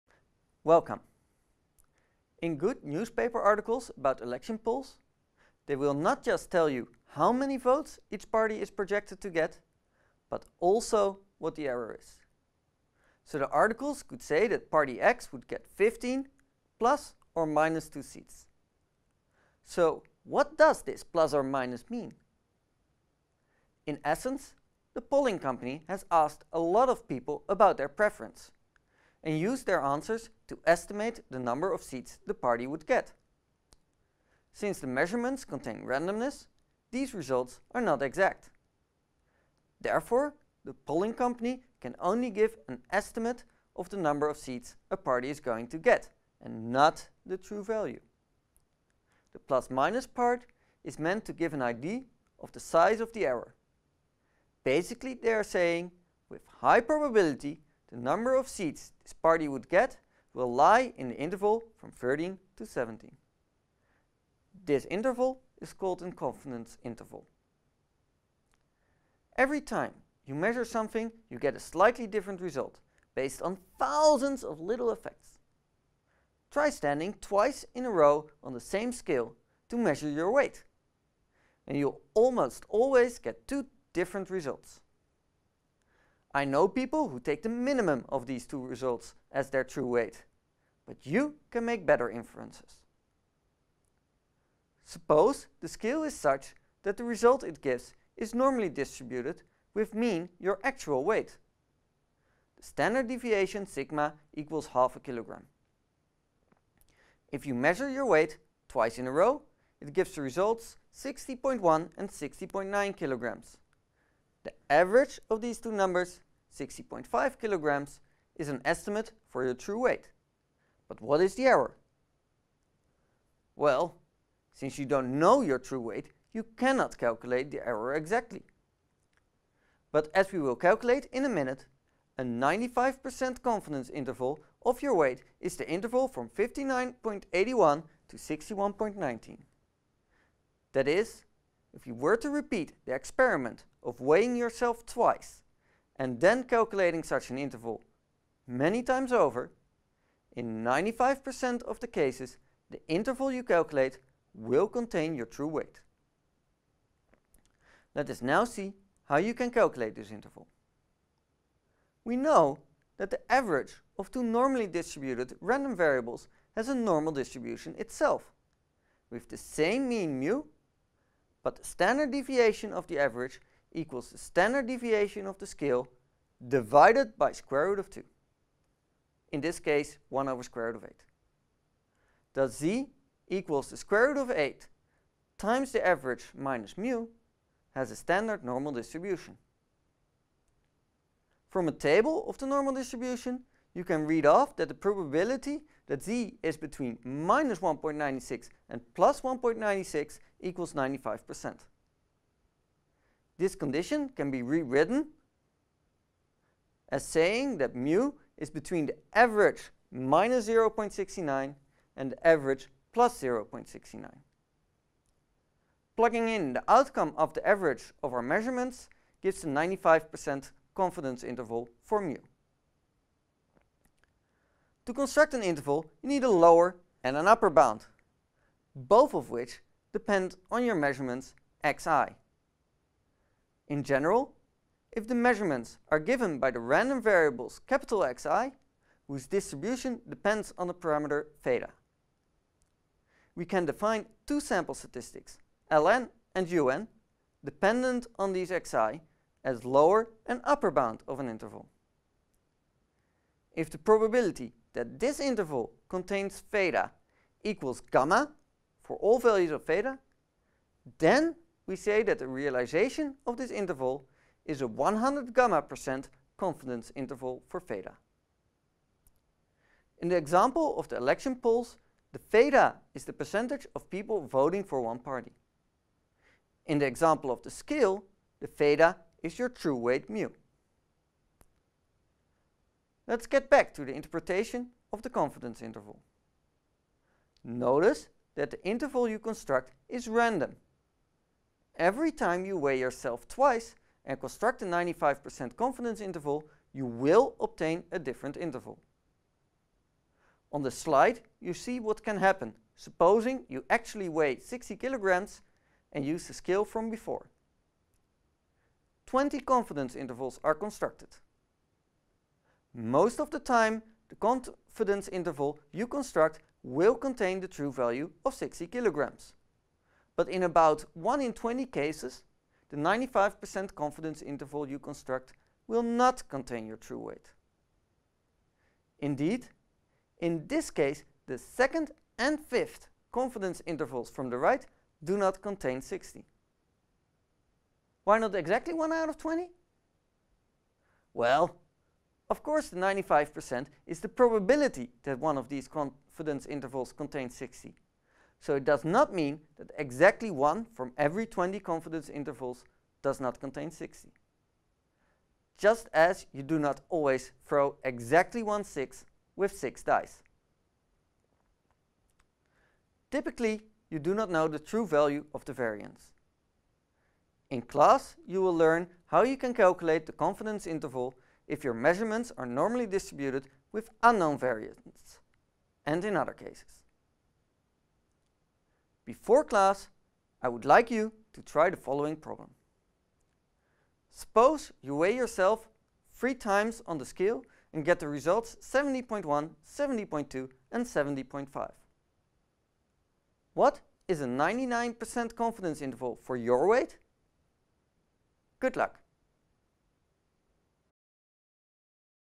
Confidence Interval for the mean - Mathematics - Probability and Statistics - TU Delft
In this video you will learn about confidence intervals with help of some examples. You will also learn how you can interpret confidence intervals and how to calculate them. This prelecture is part of the probability and statistics courses taught at TU Delft.